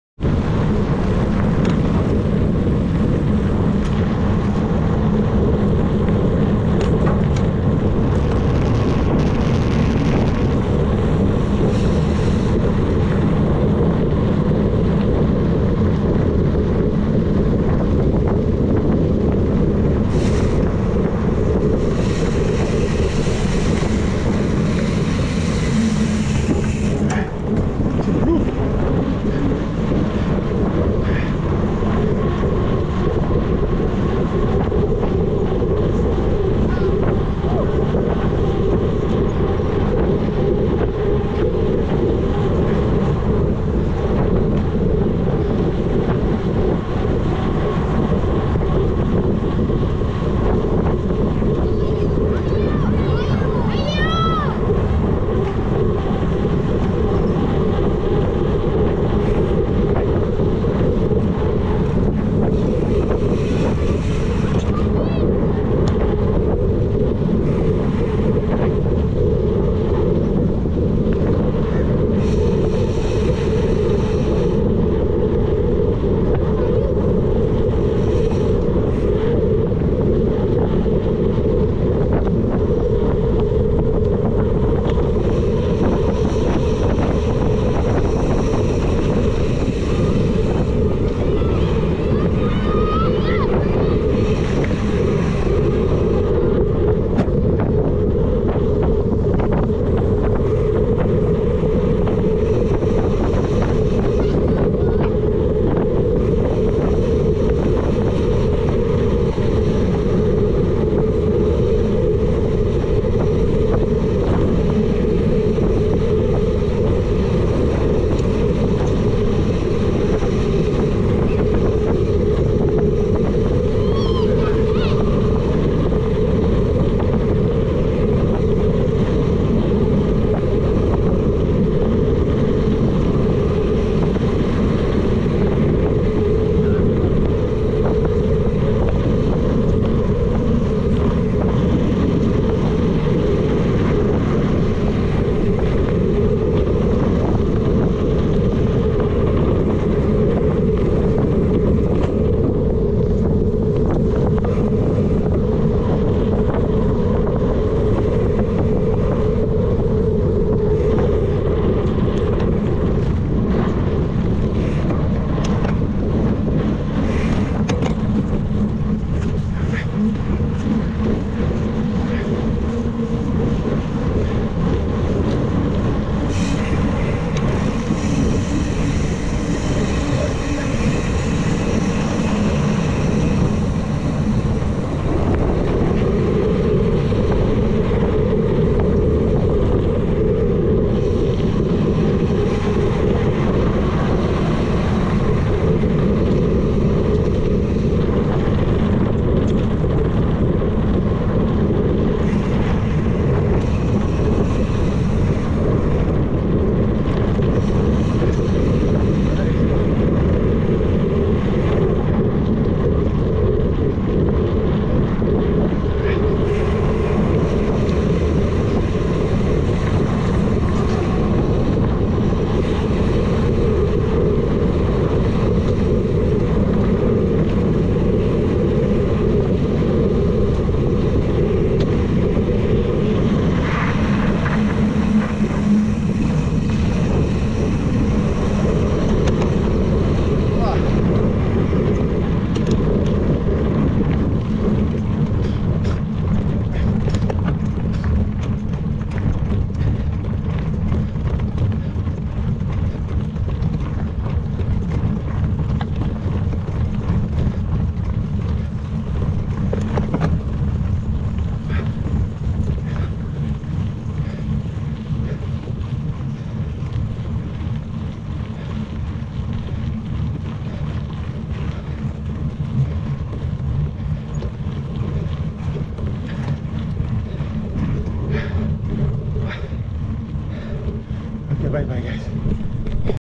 LOOP Vipingo: A Ride on the Black Mamba Chu Chu Train
I jump on the wheel of the Black Mamba train and get pulled along for a good 5 minutes. Asante sana vijana!